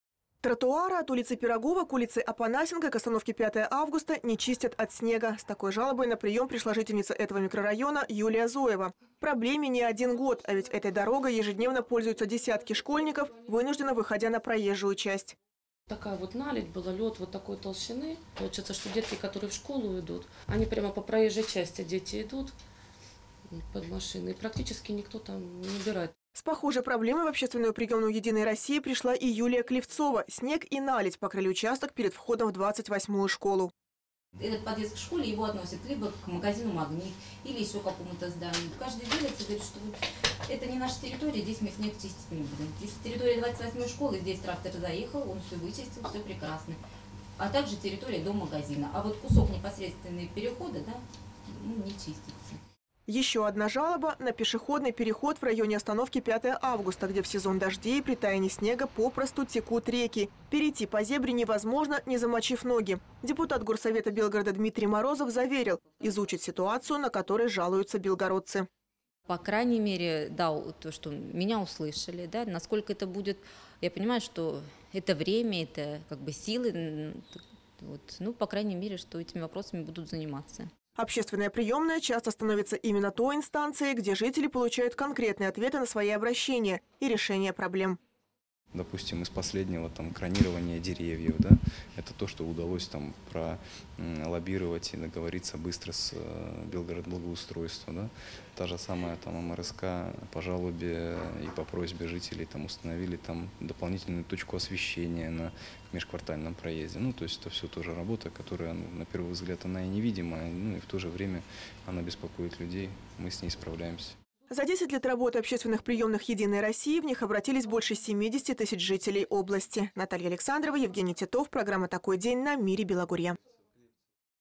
Члены «Единой России» проведут неделю встреч с гражданами
«Единая Россия» в честь своего 17-летия проводит традиционную неделю личных приёмов граждан. В этом году они будут проходить несколько дней, причём как в региональных, так и в районных общественных приёмных партии. Всего открыто более 300 дополнительных площадок в селах и хуторах. С жителями встретятся депутаты Государственной и областной Дум, муниципальных советов, земских собраний.